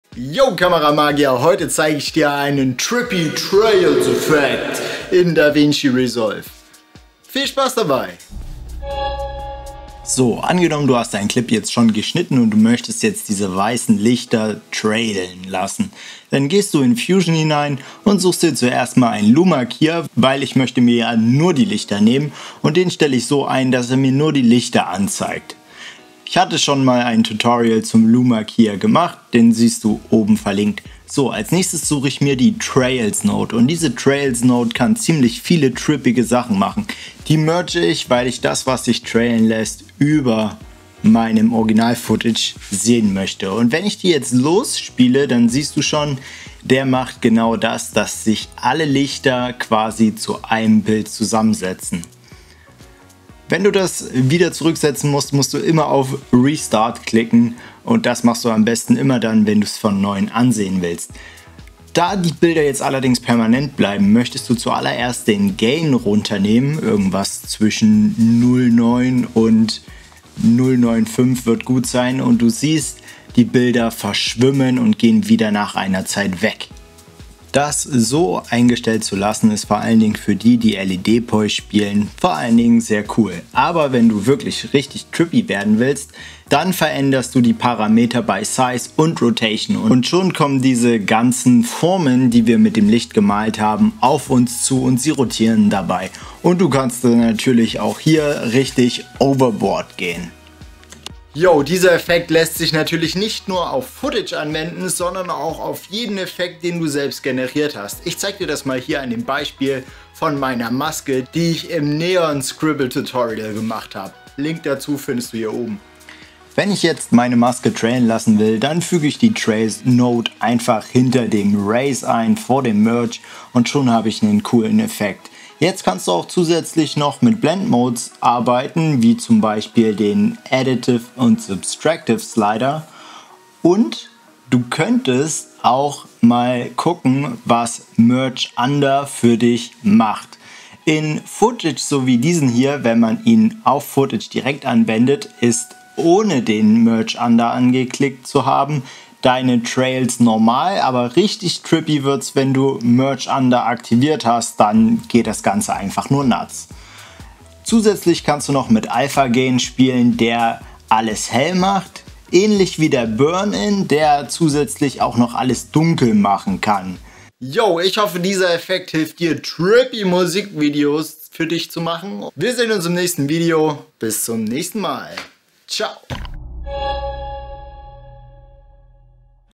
Trippy Trails Effekt - Davinci Resolve Effekte Tutorial Deutsch
Um diesen Trippy Effekt in Davinci Resolve zu nutzen brauchst du einfach nur die Trails Node. Setze den Gain am besten auf 0.95 und spiele mit Rotation und Size.

Wenn du noch tiefer in die Videobearbeitung in Davinci Resolve Fusion einsteigen willst, dann schau dir mal das Einsteigervideo an.

Wenn du bis hier unten gelesen hast, hoffe ich dass du nicht alles doppelt siehst ;)